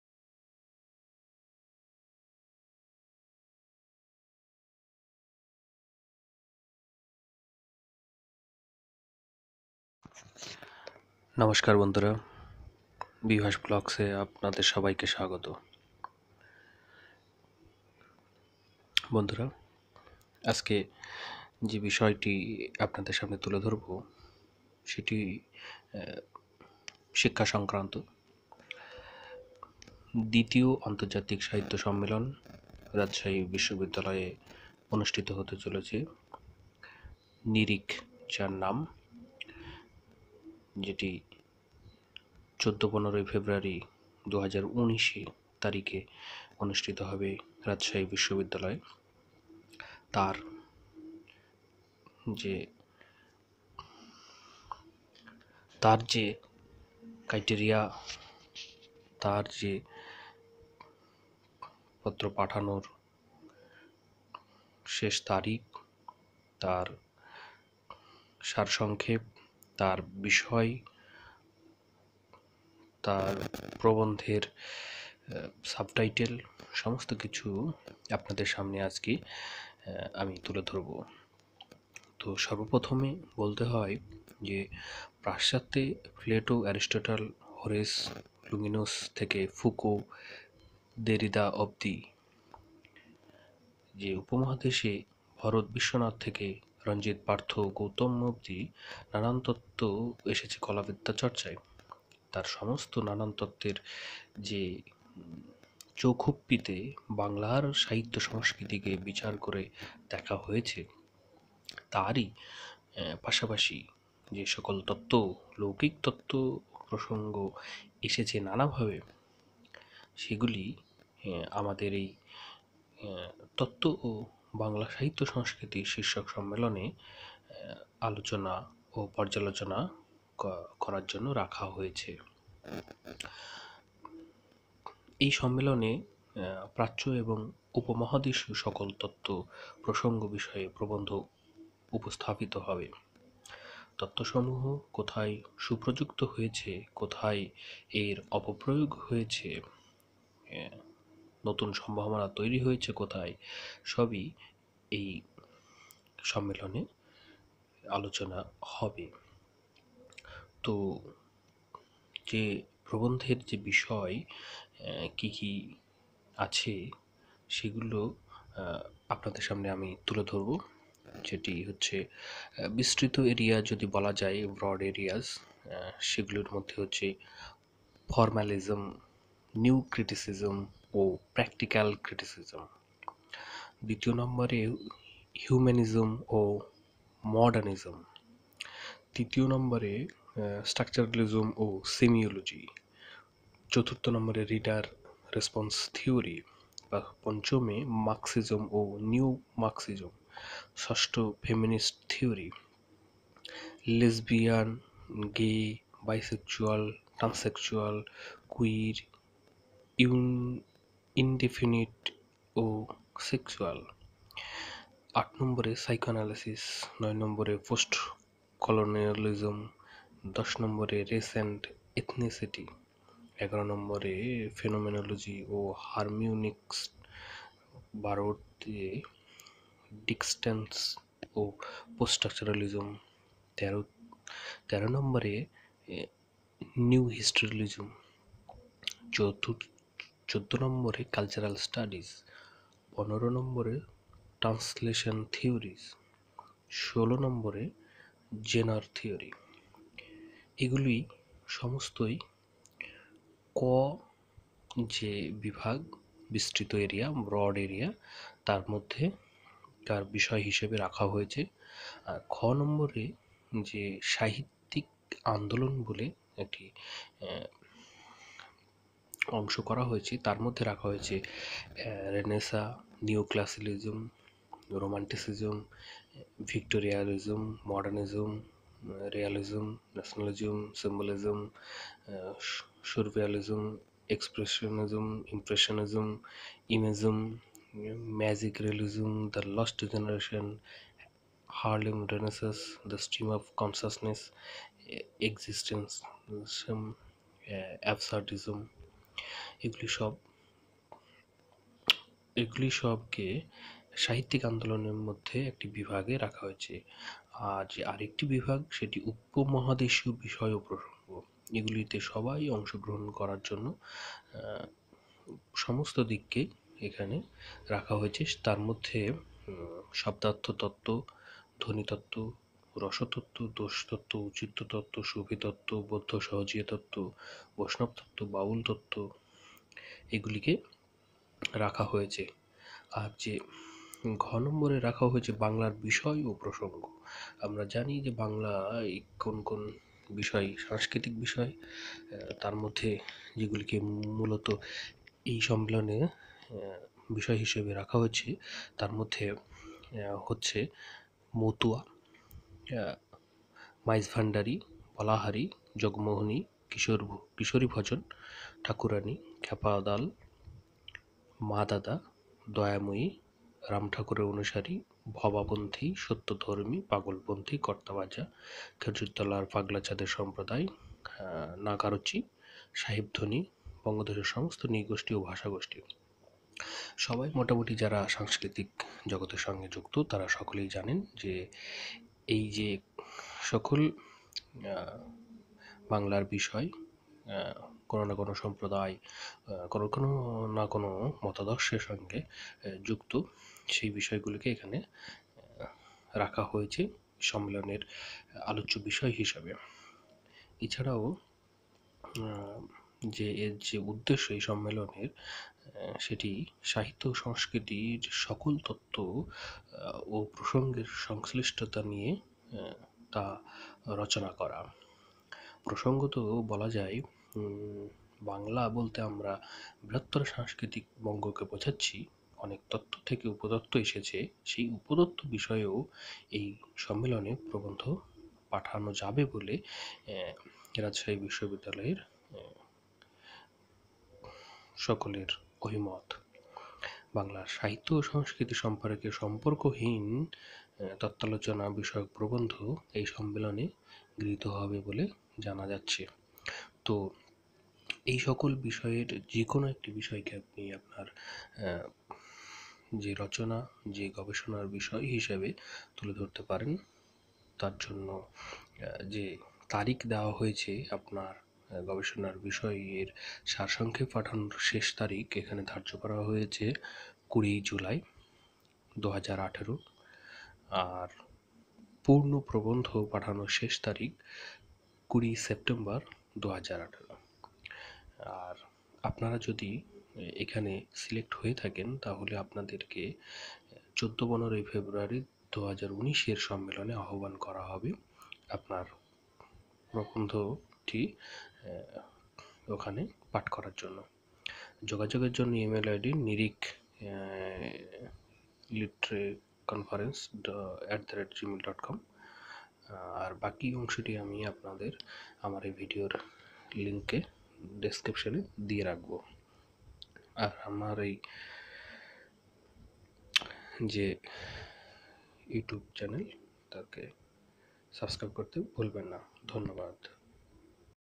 নিরিখ দ্বিতীয় আন্তর্জাতিক সাহিত্য সম্মেলন, তত্ত্ব ও বাংলার সাহিত্য-সংস্কৃতি। রাজশাহী বিশ্ববিদ্যালয়
পাশ্চাত্যে প্লেটো-এরিস্টটল-হোরেস-লুঙিনুস থেকে ফুকো- দেরিদা অব্দি এবং আমাদের উপমহাদেশে ভরত-বিশ্বনাথ থেকে রণজিৎ-পার্থ-গৌতম অব্দি নানান তত্ত্ব এসেছে কলাবিদ্যা চর্চায়। এই সকল তত্ত্ব বাংলার সাহিত্যে ও সংস্কৃতিতে প্রযুক্ত হয়েছে, প্রযুক্ত হয়েছে সাহিত্য সমালোচনার ক্ষেত্রেও। নানান তত্ত্বের চৌখুপ্পিতে বাংলার সাহিত্য-সংস্কৃতিকে বিচার করে দেখা হয়েছে। এই সকল তত্ত্বের পাশাপাশি বঙ্গদেশে লৌকিক তত্ত্ব ও প্রসঙ্গ এসেছে নানাভাবে। মধ্যপ্রাচ্যের তত্ত্ব কখনো বাংলার তত্ত্বের সাথে মিতালি করেছে; দুইয়ে মিলে নতুন তত্ত্ব সৃজিত হয়েছে। আমাদের এই ‘তত্ত্ব ও বাংলার সাহিত্য-সংস্কৃতি’শীর্ষক সম্মেলন এই সকল তত্তের আলোচনা ও পর্যালোচনা করার জন্য। সকল সময় এই সকল তত্ত্ব সাহিত্য-সংস্কৃতির ক্ষেত্রে সুপ্রযুক্ত হয়েছে একথা বলতে পারিনা।